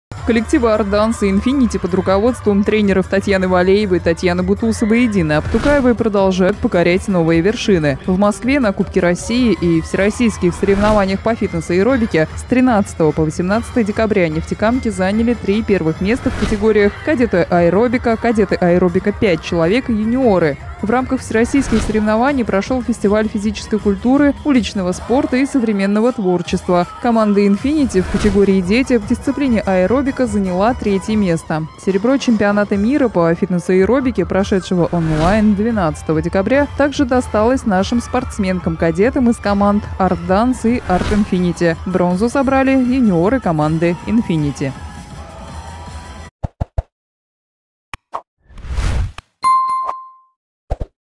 Нефтекамские спортсмены снова среди лучших
Первые места  на всероссийских соревнований по фитнес-аэробике заняли нефтекамские спортсмены. В своих возрастных категориях они стали лучшими из более чем 1000 участников.